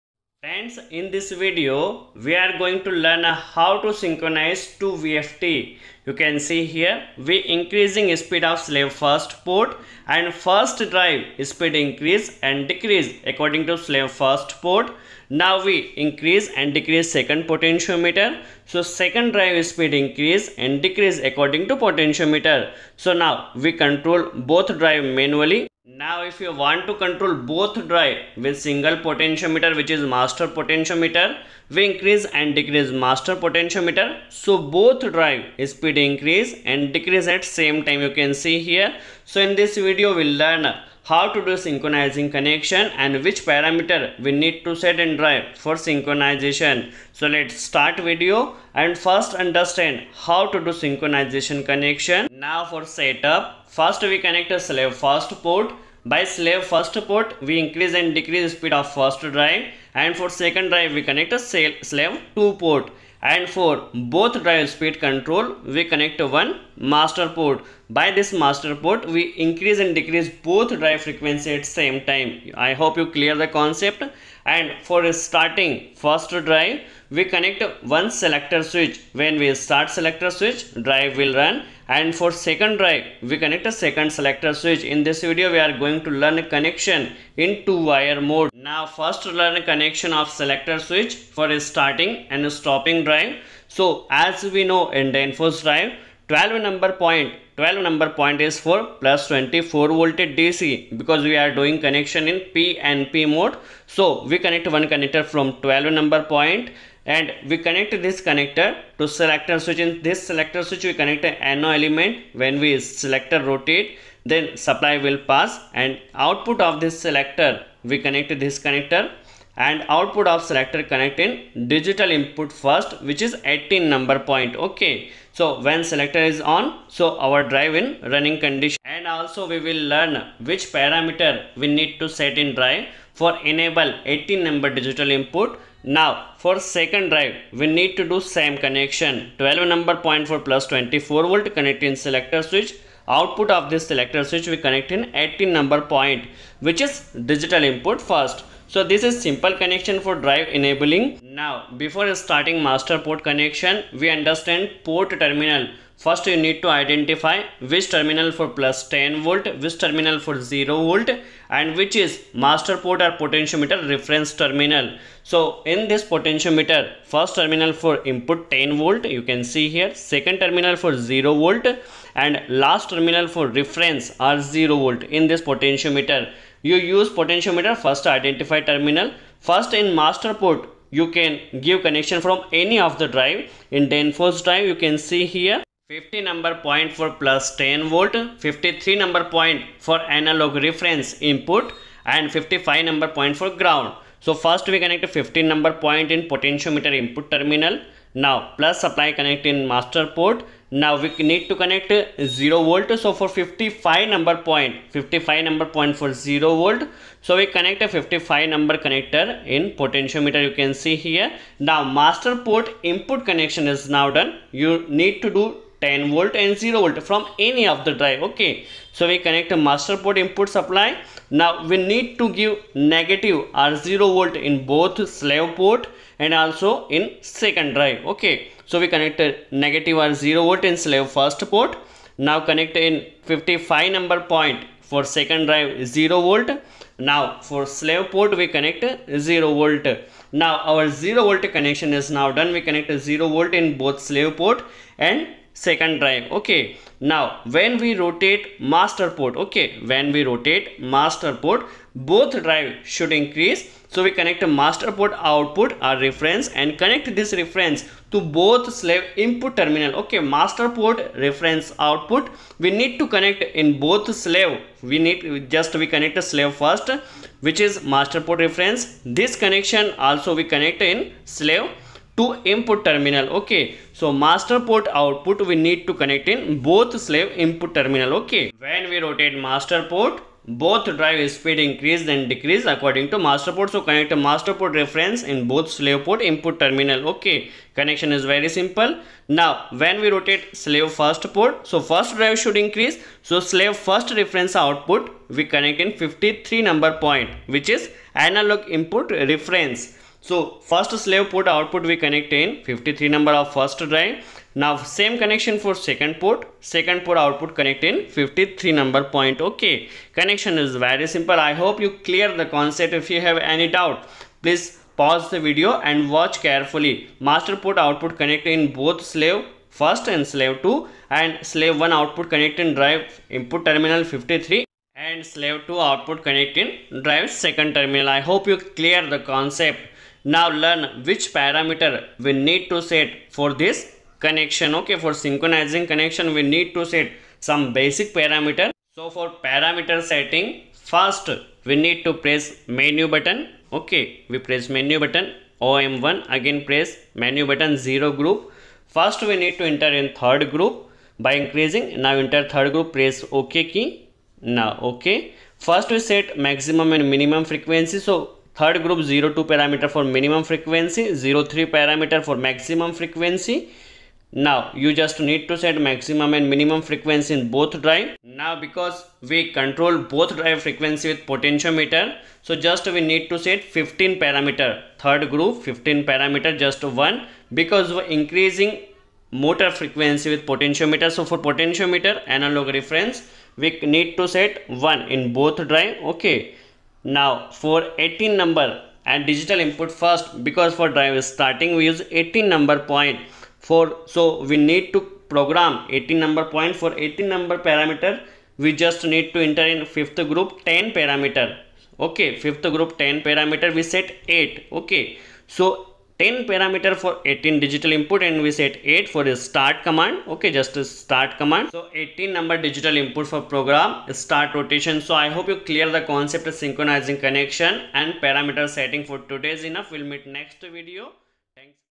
How to Synchronize Variable Frequency Drive! Vfd Synchronize Wiring and Working
in this video we explain two danfoss vfd synchronize wiring

in which we explain synchronize wiring working and parameter setting

for more information learning about drive synchronize wiring just watch and learn